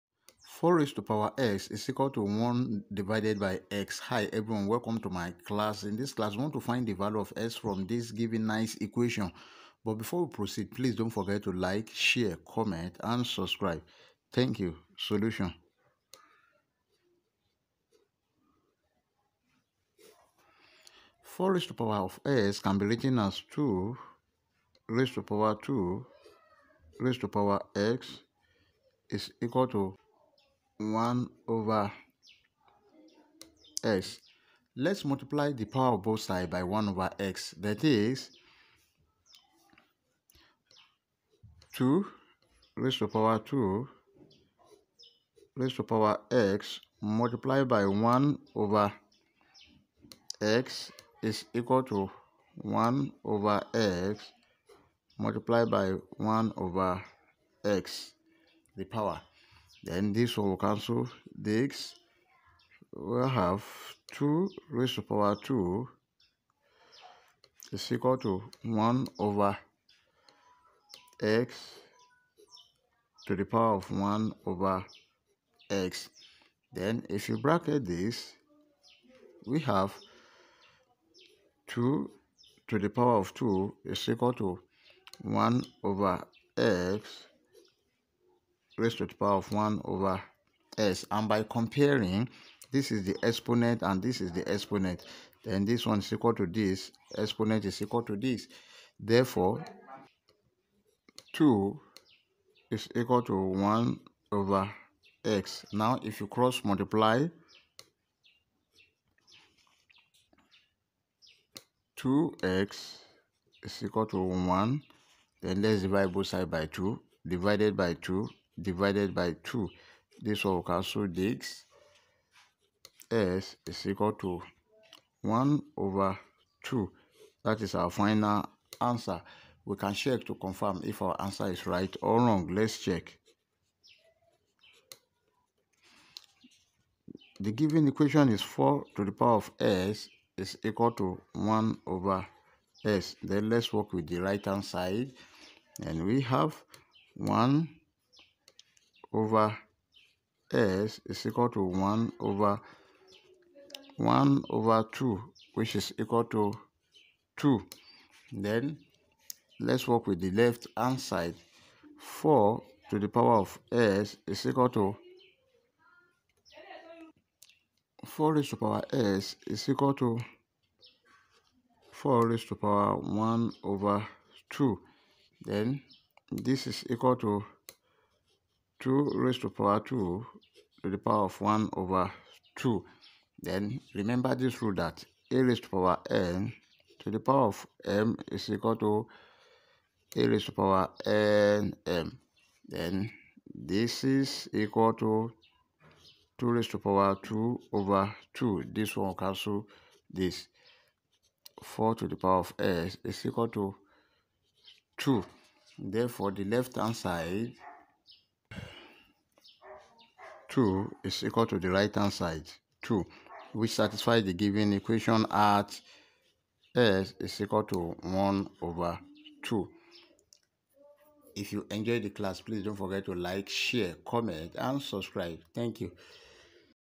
A Tricky Math Problem 4^x=1/x| How To Solve For X In This Equation?
In This video, you will learn how to solve the given nice math problem very fast.